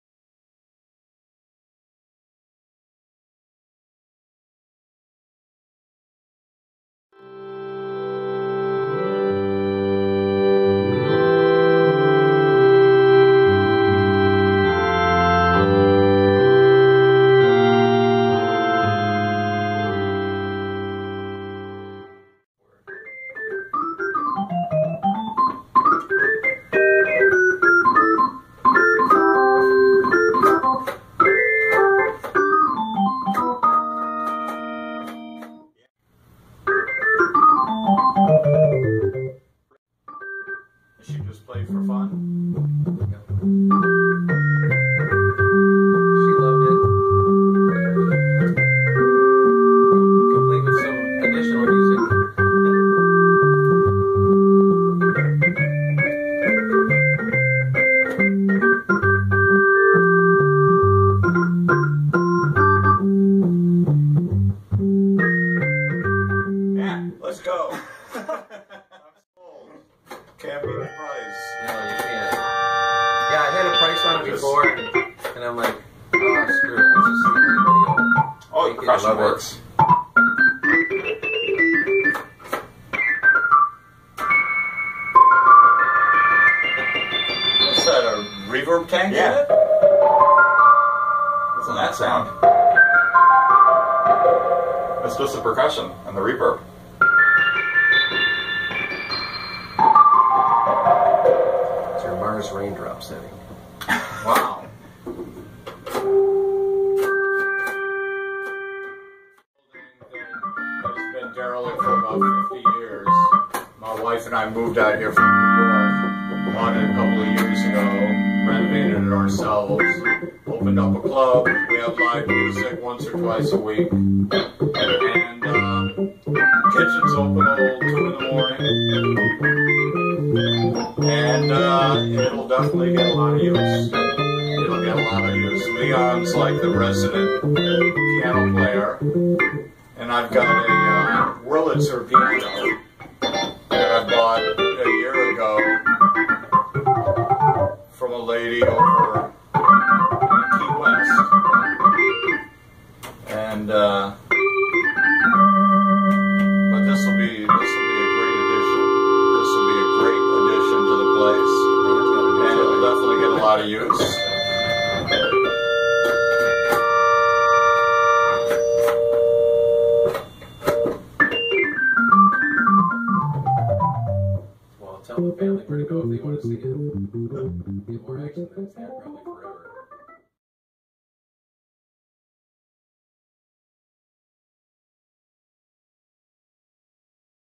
Lorraines organ
Only one person was interested in the organ and it was donated to Leon at "The Smokestack" in Dubuque that features live music.  The organ will be part of many bands as he sits in and plays along with the featured group.  I believe that Lorraine would enjoy knowing that the organ is making beautiful music again.